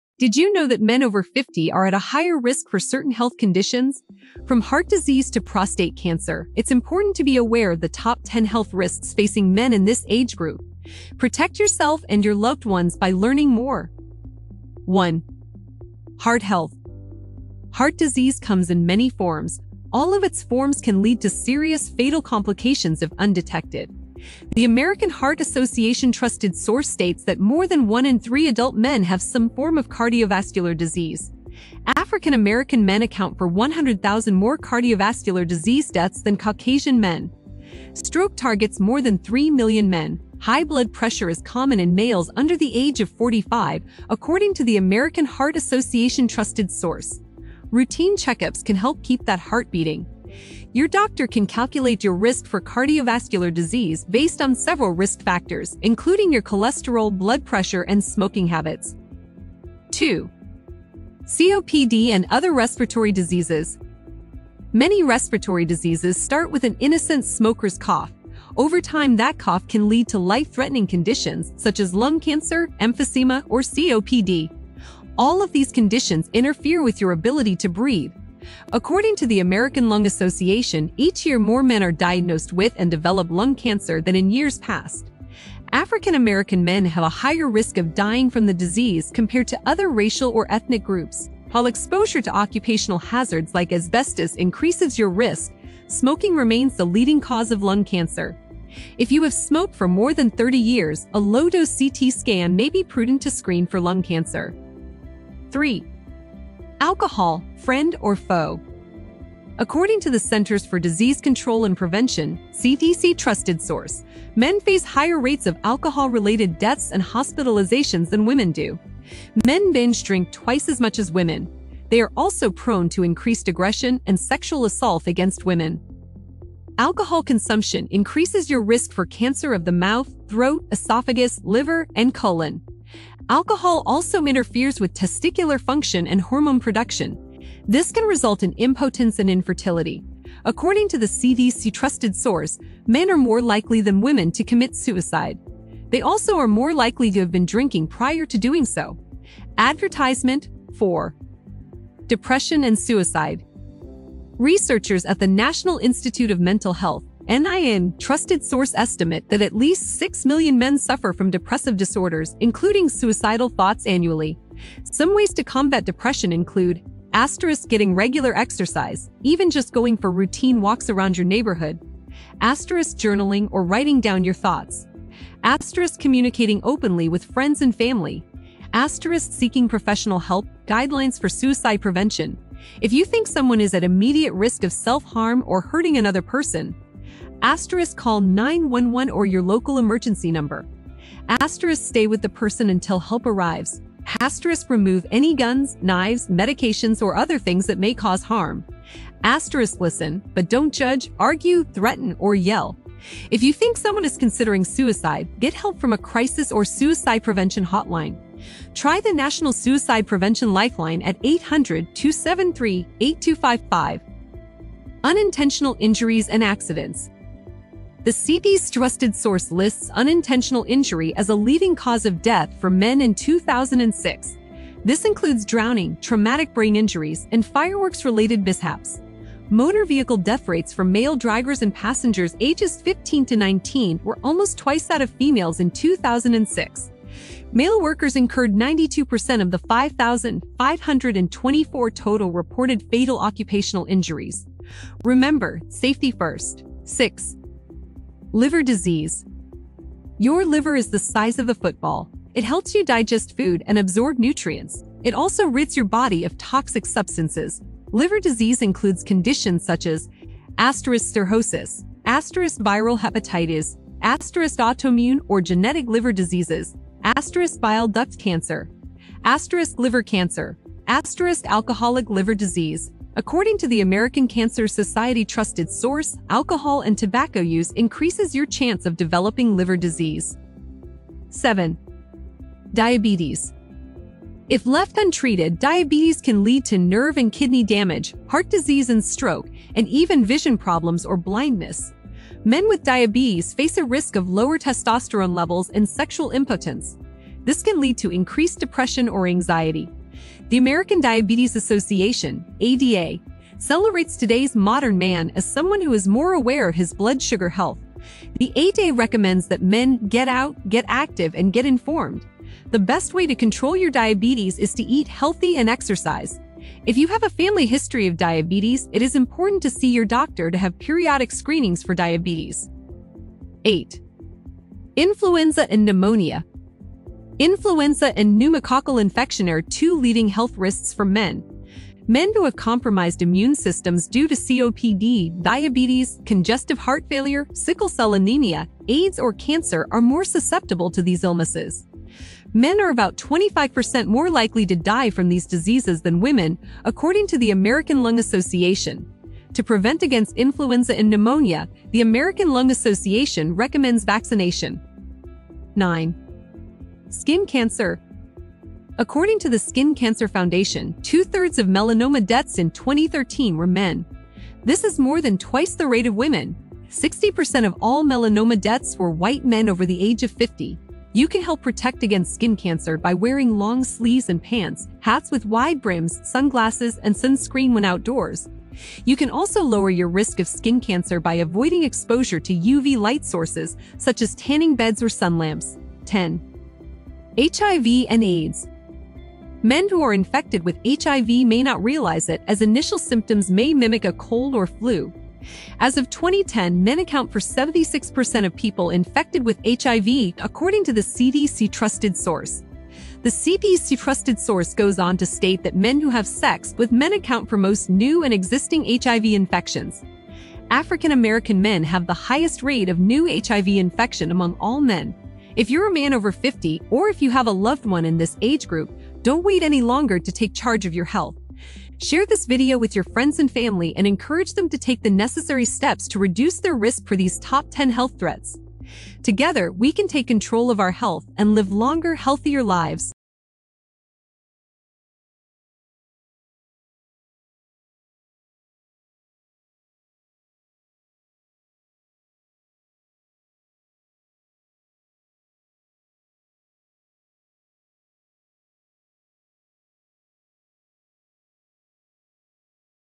Top 10 Health Risks Men Over 50 Should Be Aware Of
Are you a man over 50 and concerned about your health? In this video, we discuss the top 10 health risks that men over 50 should be aware of. From heart disease and diabetes to prostate cancer and osteoporosis, we cover the most common health concerns for men in this age group. Learn about the symptoms, causes, and ways to prevent and manage these conditions to keep yourself healthy and happy. Watch now and stay informed about the health risks that matter most to you!